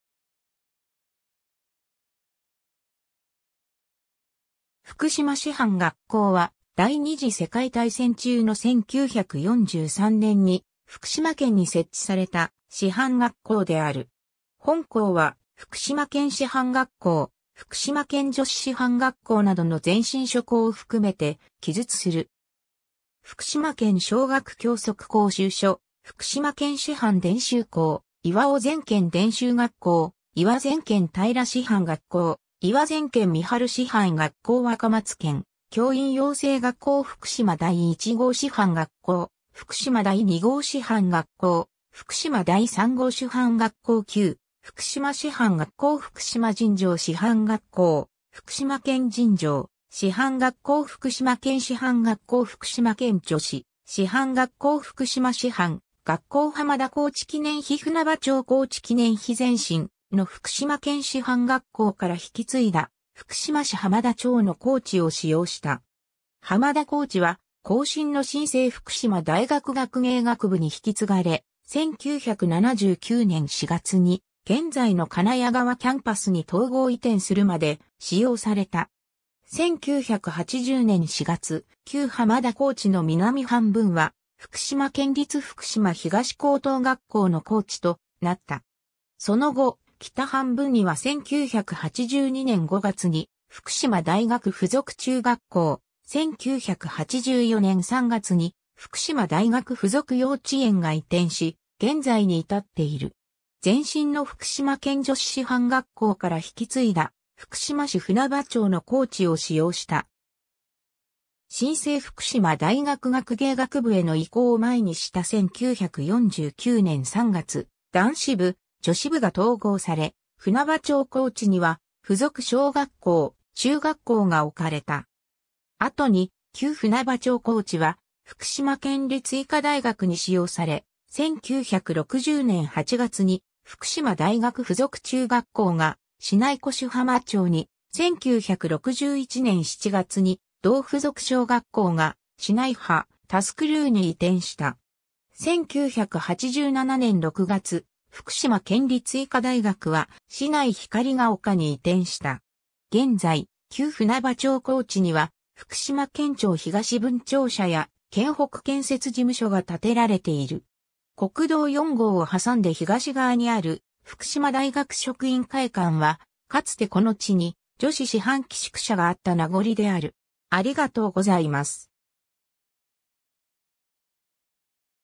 福島師範学校
福島師範学校（ふくしましはんがっこう）は、第二次世界大戦中の1943年（昭和18年）に福島県に設置された師範学校である。
 本項は、福島県師範学校・福島県女子師範学校などの前身諸校を含めて記述する。
 福島県小学教則講習所、福島県師範伝習校 磐前県伝習学校、磐前県平師範学校、磐前県三春師範学校 若松県教員養成学校 福島第一号師範学校、福島第二号師範学校、福島第三号師範学校 旧・福島師範学校 福島尋常師範学校、福島県尋常師範学校 福島県師範学校 福島県女子師範学校 福島師範学校 浜田校地記念碑 舟場町校地記念碑 前身の福島県師範学校から引き継いだ福島市浜田町の校地を使用した。
浜田校地は後身の新制福島大学学芸学部（1966年教育学部と改称）に引き継がれ、1979年4月に現在の金谷川キャンパスに統合移転するまで使用された。
1980年4月、旧浜田校地の南半分は福島県立福島東高等学校の校地となった。
その後、北半分には1982年5月に福島大学附属中学校、1984年3月に福島大学附属幼稚園が移転し、現在に至っている。
 前身の福島県女子師範学校から引き継いだ福島市舟場町（現・杉妻町）の校地を使用した。
新制福島大学学芸学部への移行を前にした 1949年3月、男子部・女子部が統合され、舟場町校地には附属小学校・中学校が置かれた。
後に、旧舟場町校地は福島県立医科大学に使用され、1960年8月に福島大学附属中学校が市内腰浜町に、1961年7月に同附属小学校が市内歯扶柳（現・新浜町）に移転した。
1987年6月、福島県立医科大学は市内光が丘に移転した。
現在、旧舟場町校地には、福島県庁東分庁舎や県北建設事務所が建てられている。
国道4号を挟んで東側にある福島大学職員会館は、かつてこの地に女子師範寄宿舎（後の葵寮）があった名残りである。